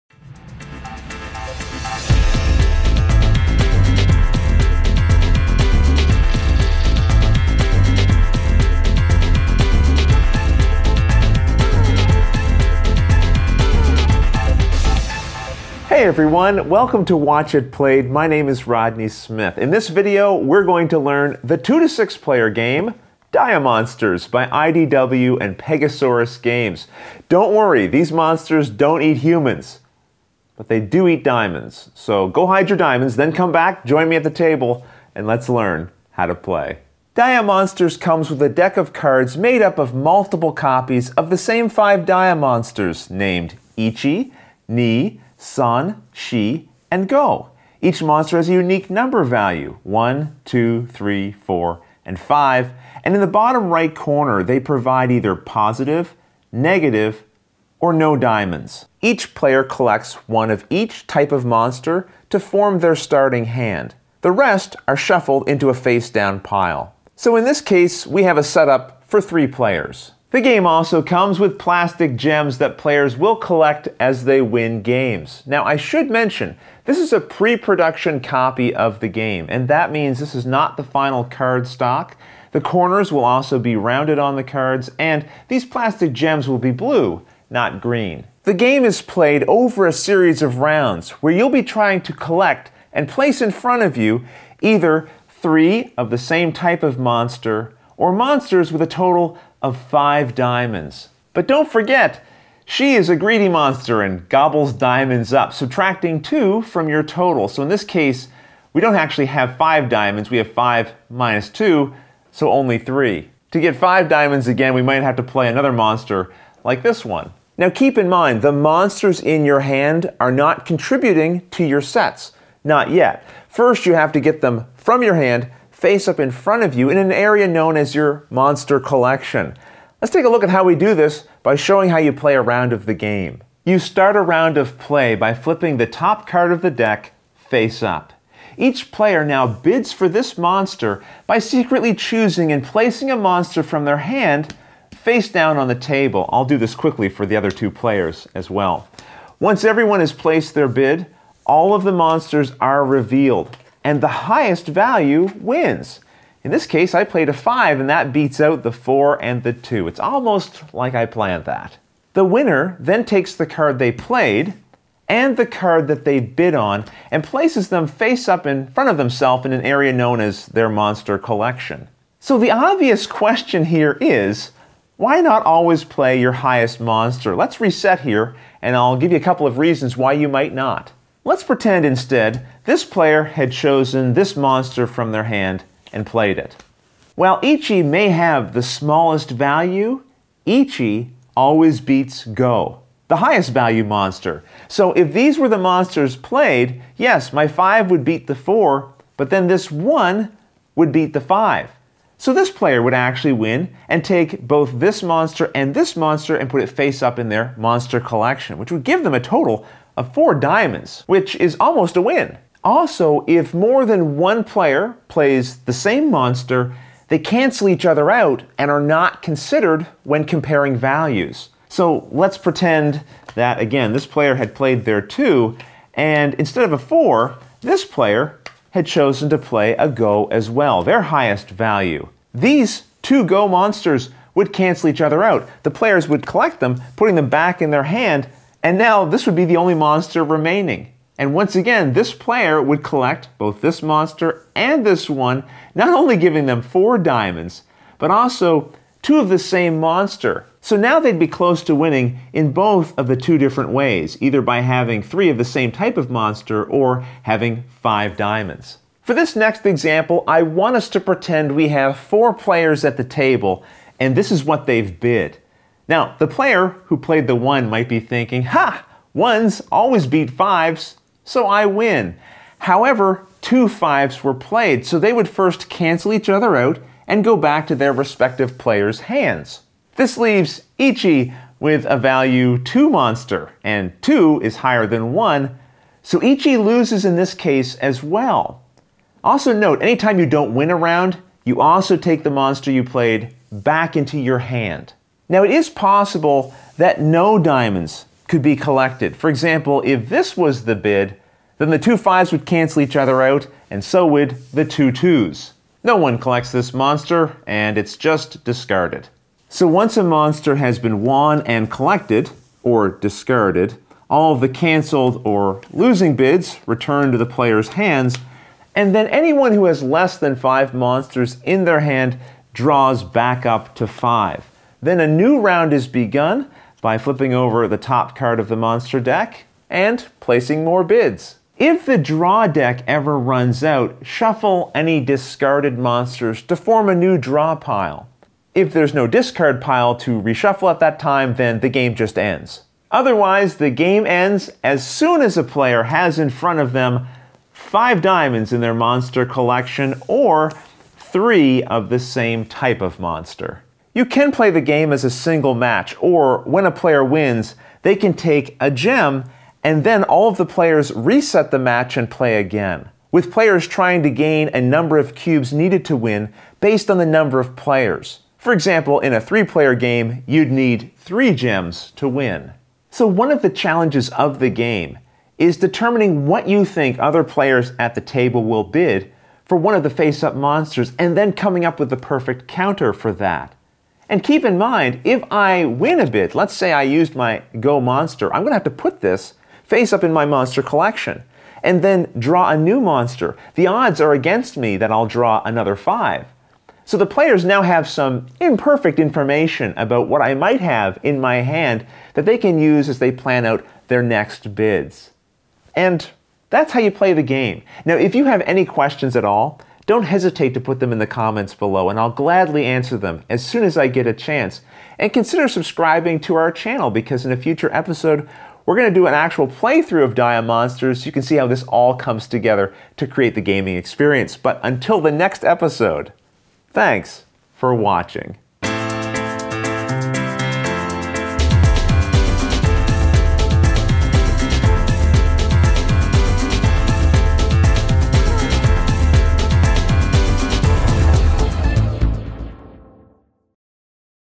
Diamonsters - How To Play
In this video, we're going to learn how to play Diamonsters! If you have any comments or questions, please do not hesitate to post them in the YouTube comments below.

00:00 - Introduction
00:37 - The Setup
01:28 - Game Play Overview
02:14 - A Round Of Play
02:51 - Monster Interactions
05:42 - Ending The Game
06:12 - Imperfect Information
06:46 - Conclusion